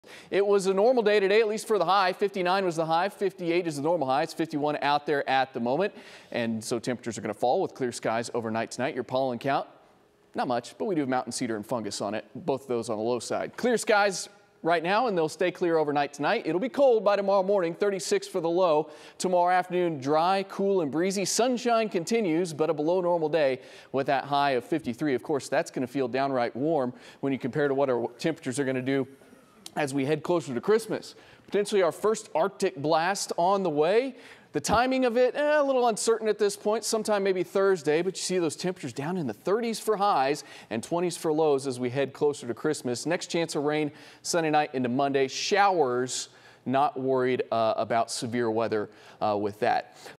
DFW weather: Arctic air for Christmas?
It's going to be a chilly Christmas holiday, if the forecast holds.